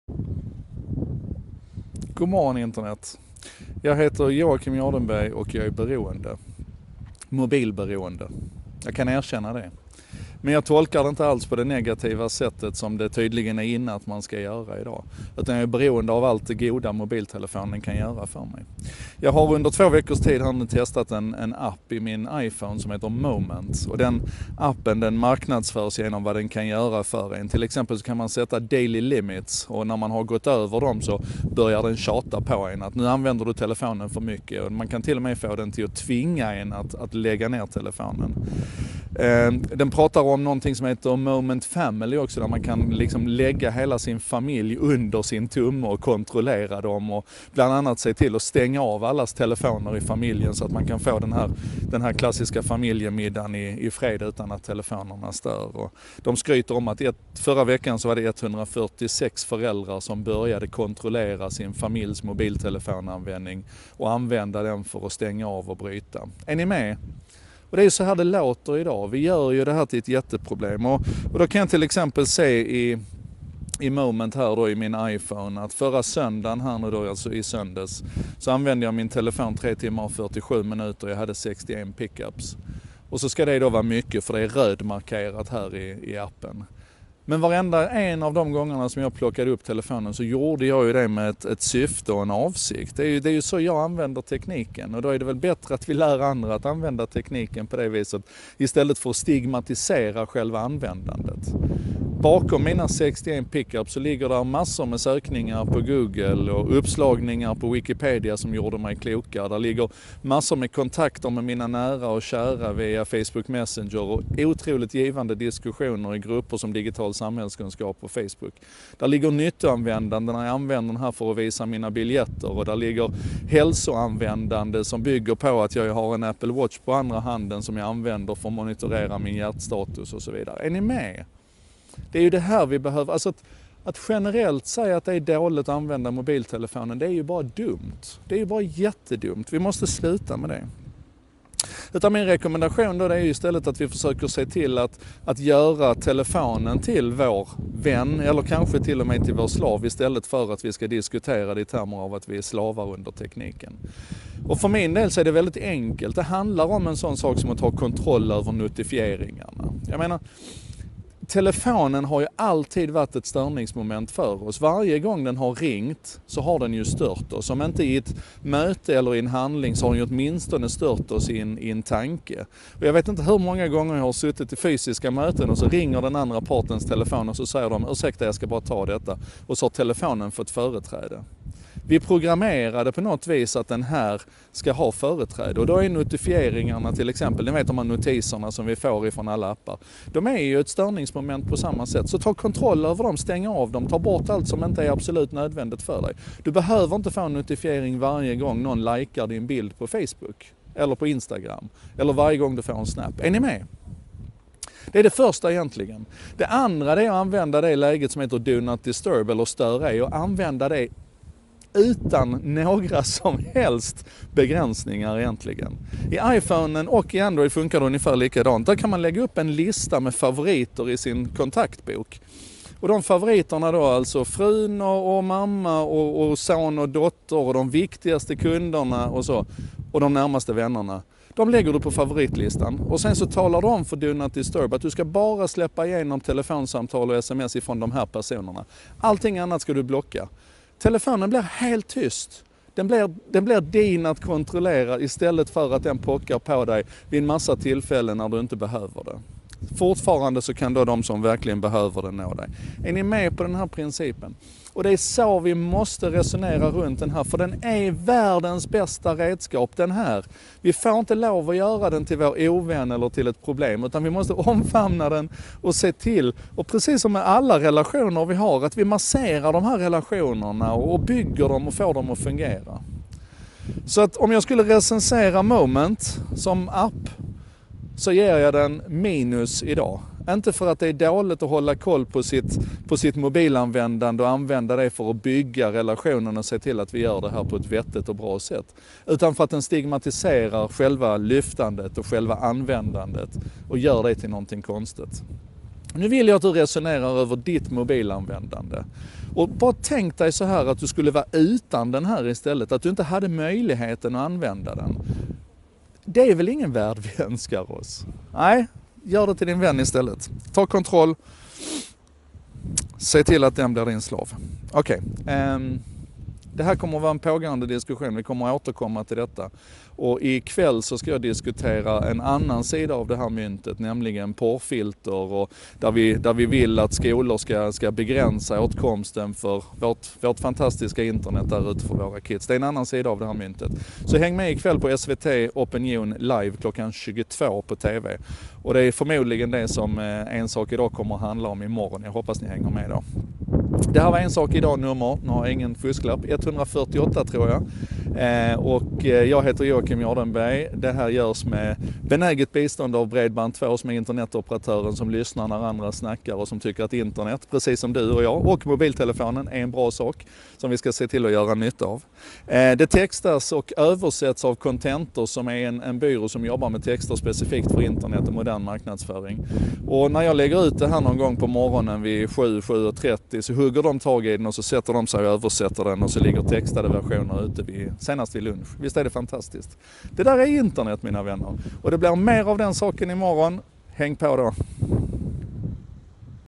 148. Ja, jag är mobilberoende
Jag heter Joakim och jag är mobilberoende. Och jag tycker det är alldeles fantastiskt.